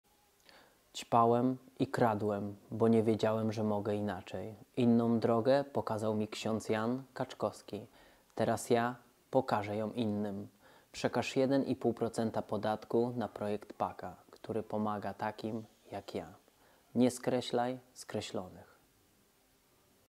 ks. Jan Kaczkowski i Patryk Galewski - przekaż 1,5% na projekt PaKa
Patryk Galewski - bohater filmu "JOHNNY".
Dzięki ks. Janowi Kaczkowskiemu otrzymał nowe życie.
Teraz razem uratujmy kolejne.

Ćpałem i kradłem, bo nie wiedziałem, że mogę inaczej.
Inną drogę pokazał mi ks. Jan Kaczkowski.
Teraz ja pokażę ją innym.
Przekaż 1,5% podatku na projekt PaKa.
NIE SKREŚLAJ SKREŚLONYCH.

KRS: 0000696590